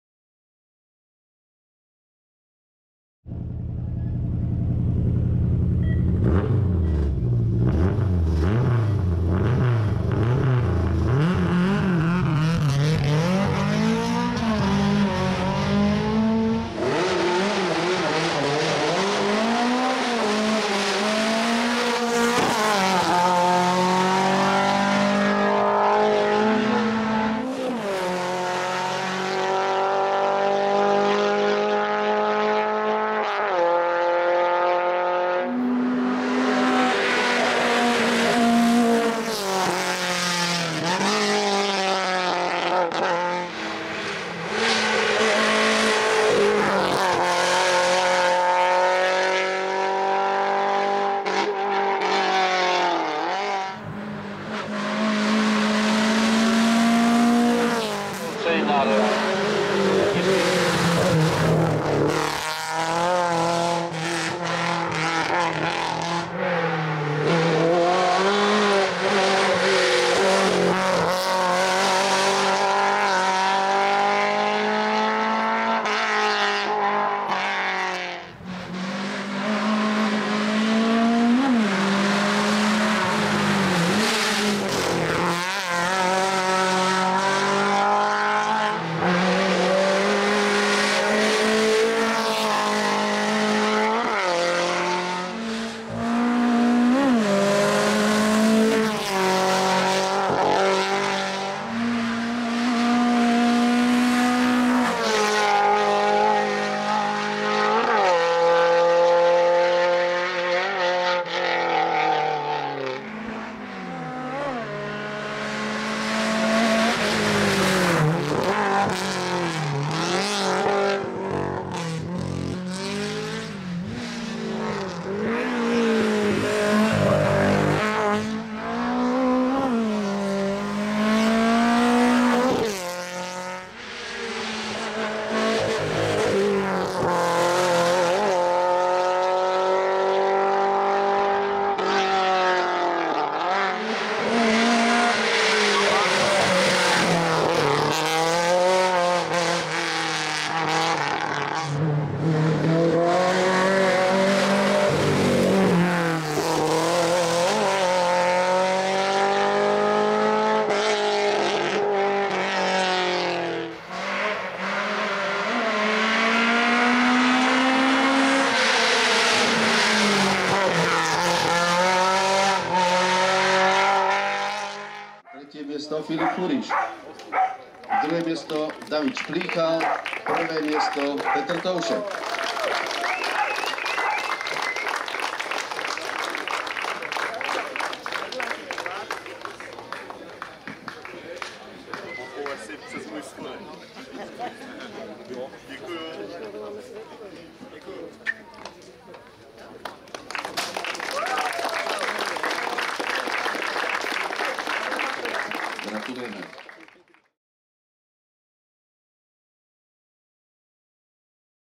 VW Golf GTI - David Šplíchal - XVI.Divácká 13 - Diváky u Hustopečí 2021
David Šplíchal si s vozem VW Golf GTI dojel po oba víkendové závody pro druhé místo ve třídě HA1-2000 v závodech do vrchu XVI.Divácká 13 v Divákách u Hustopečí.